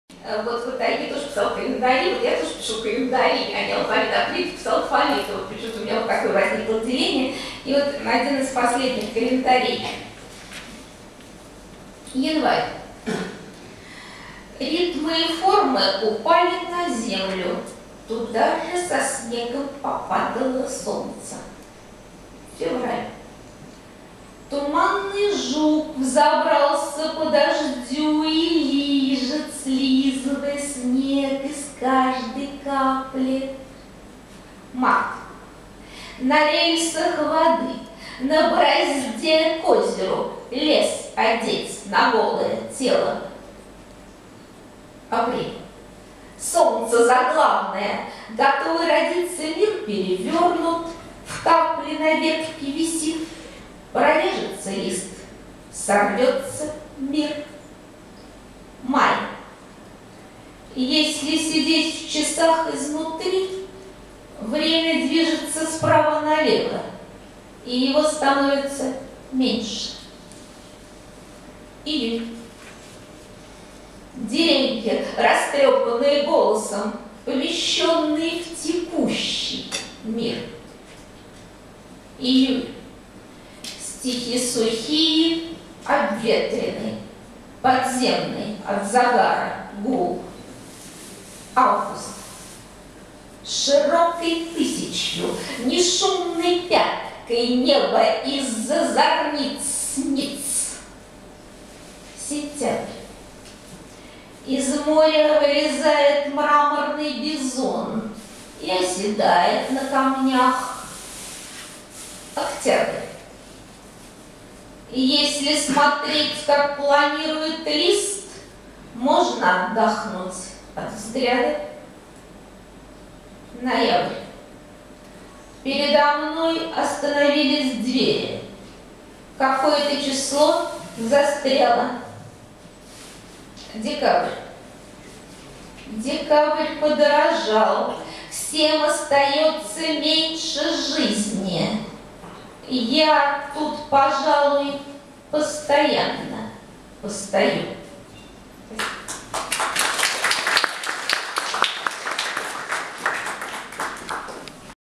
Наталия Азарова. Стихотворение Календарь
Наталия Азарова на фестивале современной поэзии "ГолосА" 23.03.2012. Чебоксары, Национальная библиотека Чувашии